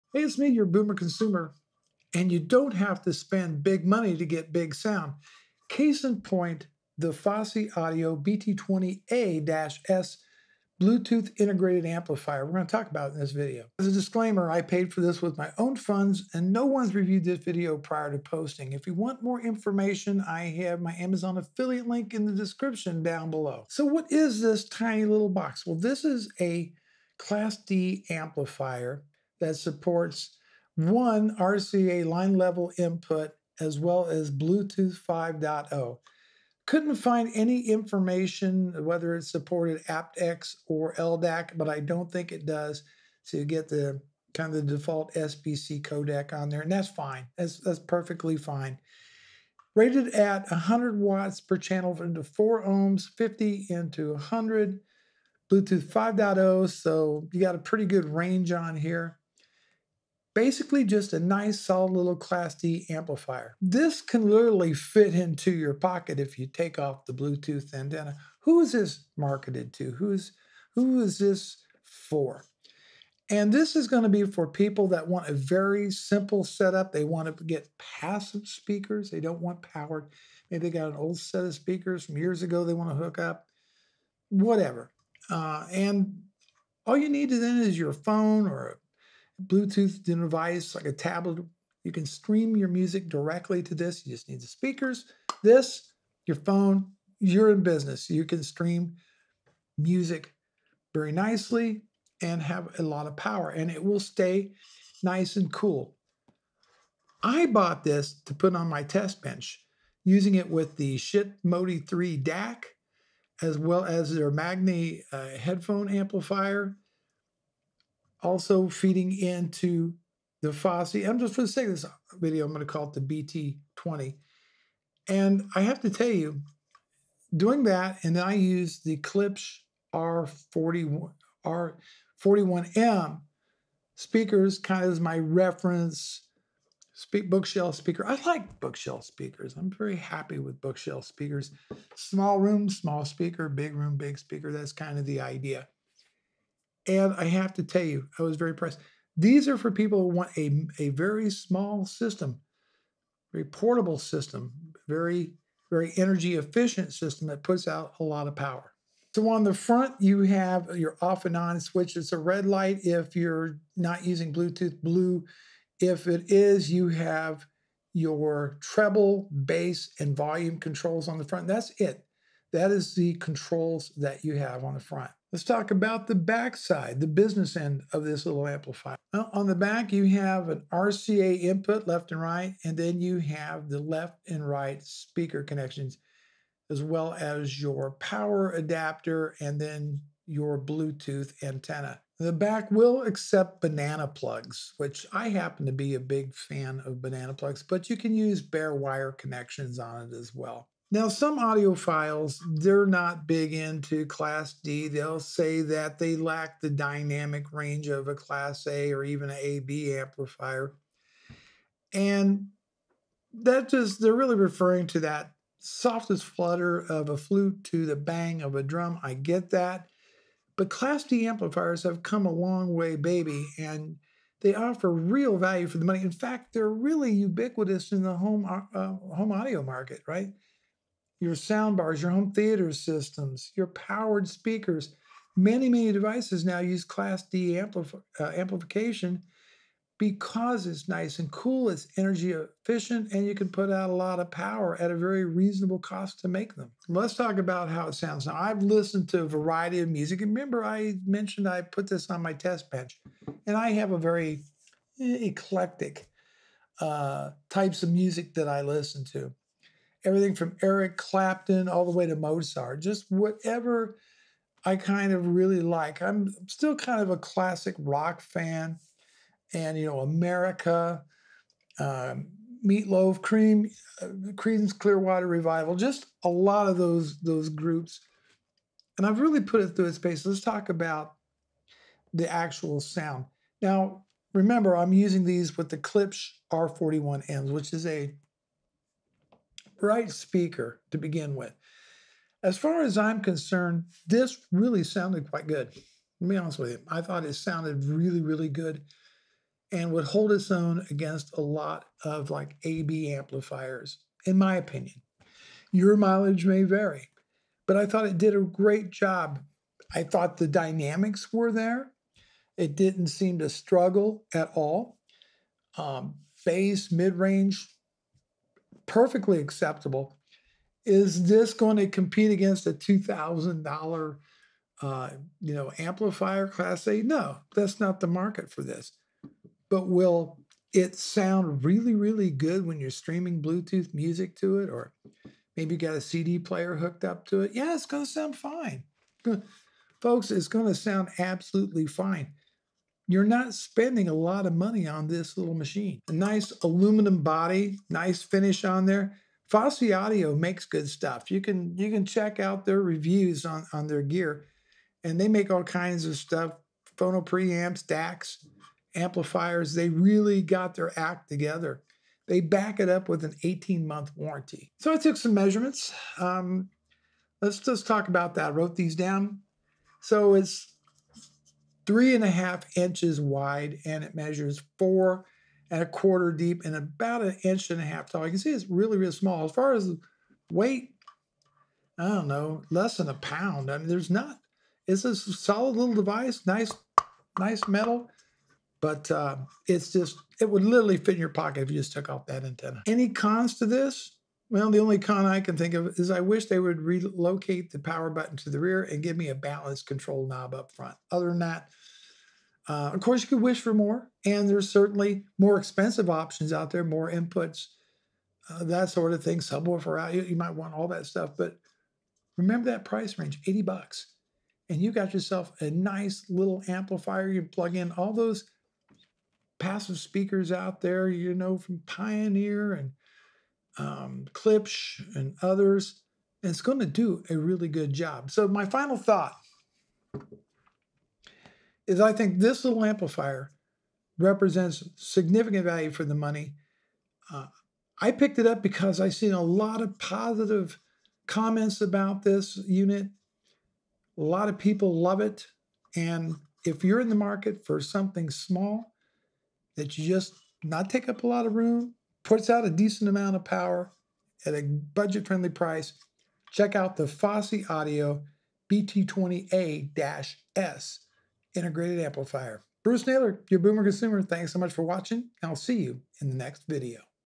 Big Sound in a Small Package! Fosi Audio BT20A-S Bluetooth Amp Review
The Fosi Audio BT20A-S Class D amp is a budget champ mini amplifier.

Amazon Store

Cameras

Lenses

Microphone

Lights

Voice Over Microphones

Audio Technica ATR2100
Audio Technica AT2020

USB Audio Interface

Social